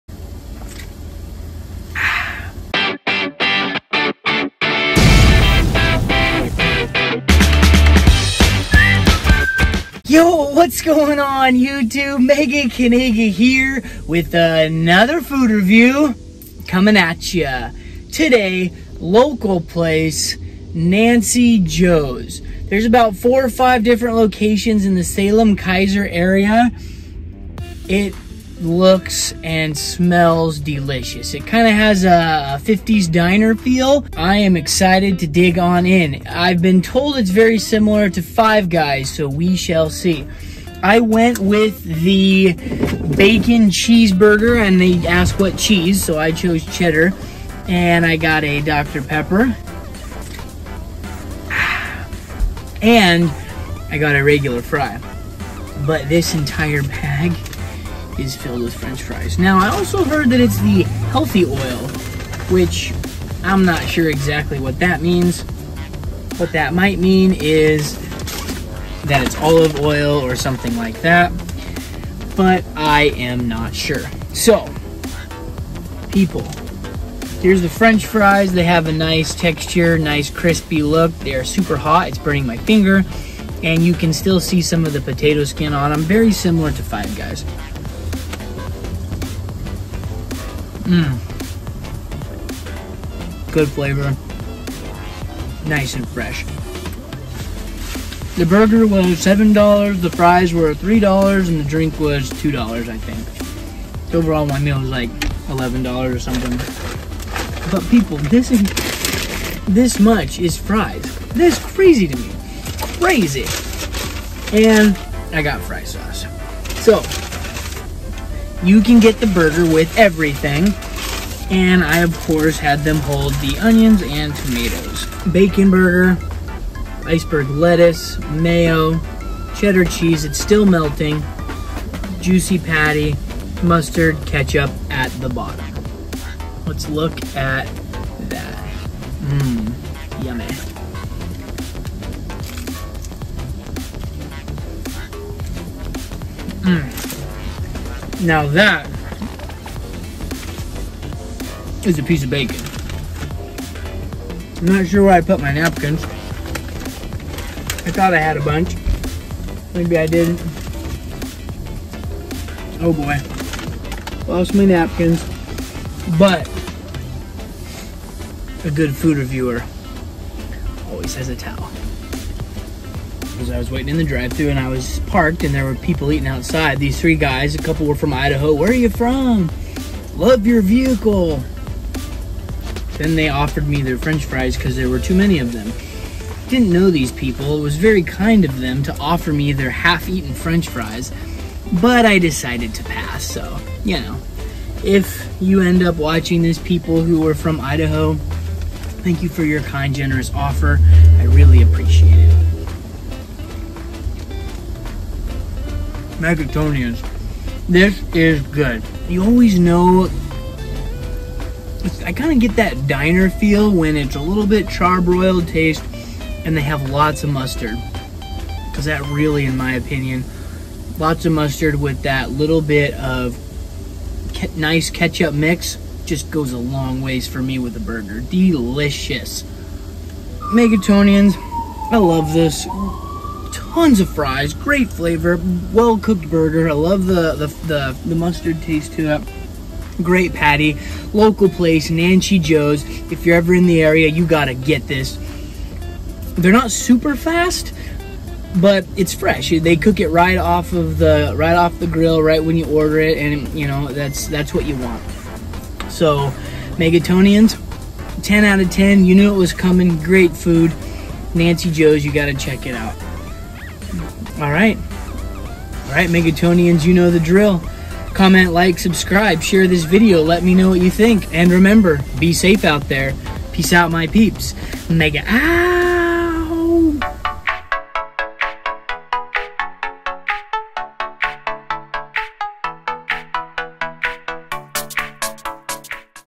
Local: Nancy Jo's Burgers and Fries | MegaCanaga
Calories Episode-Food Review: Nancy Jo's Burgers and Fries
Rating: 10/10

That Was A Ton Of Fries!!!

God Bless